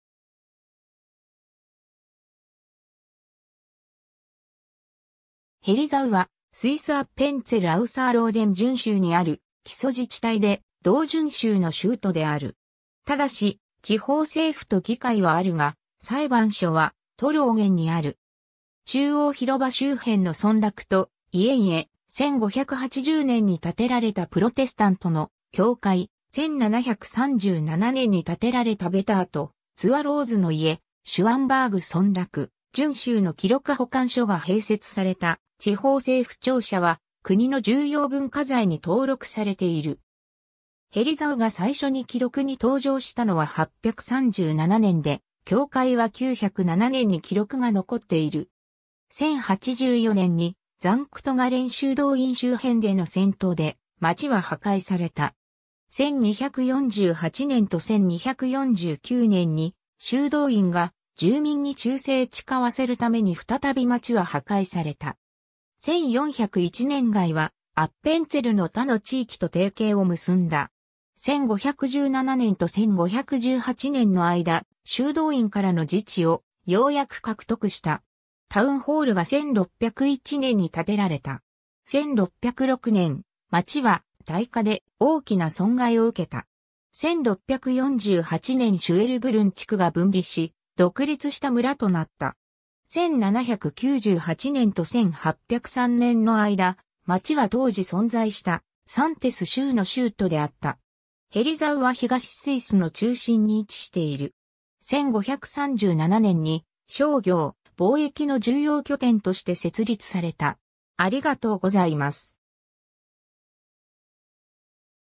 ヘリザウ
アッペンツェル・アウサーローデン準州の基礎自治体
ヘリザウ（Herisau）は、スイス・アッペンツェル・アウサーローデン準州にある基礎自治体 ( ベツィルク ) で、同準州の州都である。
ただし地方政府と議会はあるが、裁判所はトローゲンにある。
 中央広場周辺の村落と家々、1580年に建てられたプロテスタントの教会、1737年に建てられたWetterとzur Roseの家、Schwänberg 村落、準州の記録保管所が併設された地方政府庁舎は国の重要文化財に登録されている。
 ヘリザウが最初に記録に登場したのは837年で、教会は907年に記録が残っている。
1084年にザンクト・ガレン修道院周辺での戦闘で、街は破壊された。
1248年と1249年に修道院が住民に忠誠誓わせるために再び街は破壊された。
1401年街はアッペンツェルの他の地域と提携を結んだ。
 1517年と1518年の間、修道院からの自治をようやく獲得した。
タウンホールが1601年に建てられた。
1606年、街は大火で大きな損害を受けた。
1648年Schwellbrunn地区が分離し、独立した村となった。
1798年と1803年の間、街は当時存在したSäntis州の州都であった。
 ヘリザウは東スイスの中心に位置している。
1537年に商業・貿易の重要拠点として設立された。